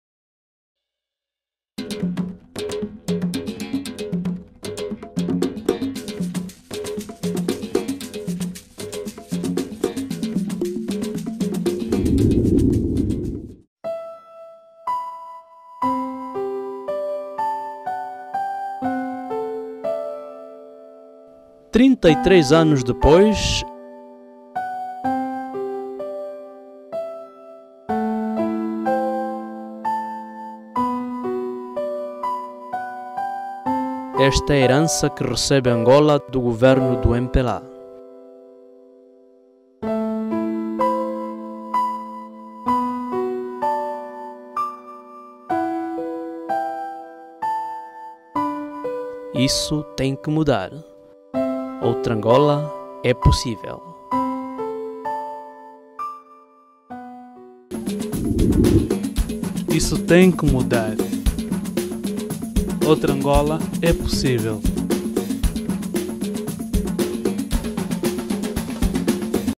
UNITA (Tristeza)
3 anos depois, esta é a herança que recebe Angola do governo do MPLA. Teu voto, pode mudar o nosso destino.